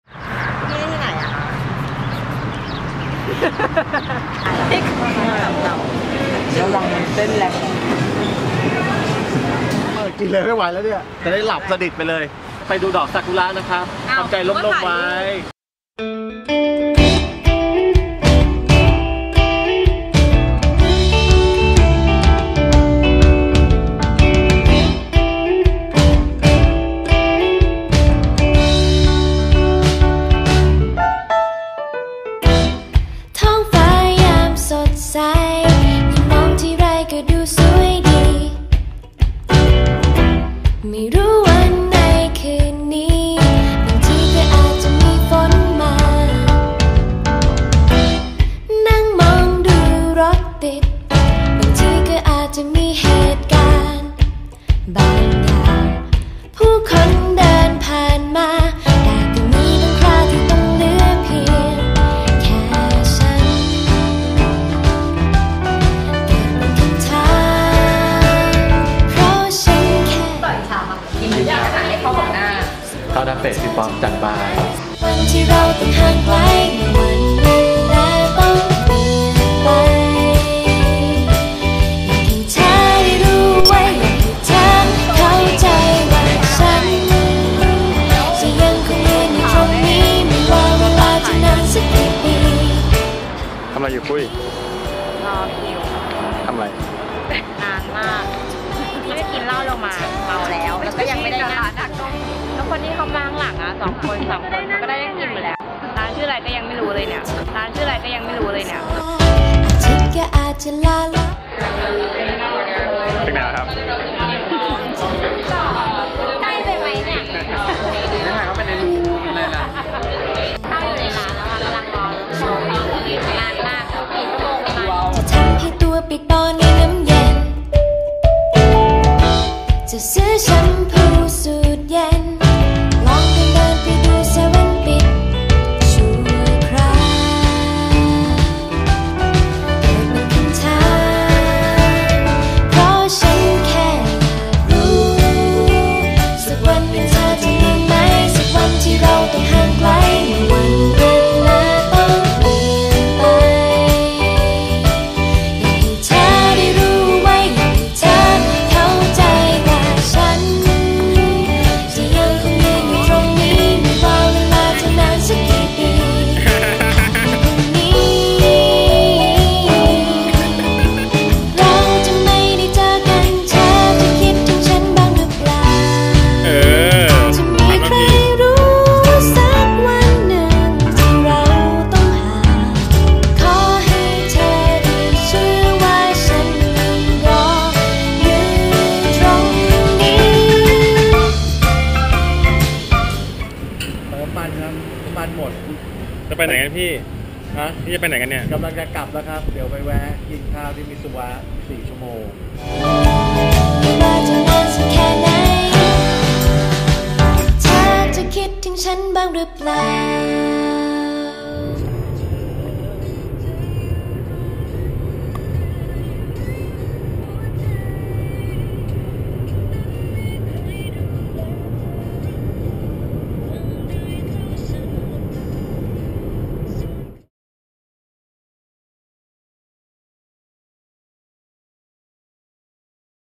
DC Cherry blossoms 2014
A road trip journal Boston to DC.

ไฟล์น้อย ๆ. ทำเก็บไว้ดูตอนแก่กันนะจ๊ะ.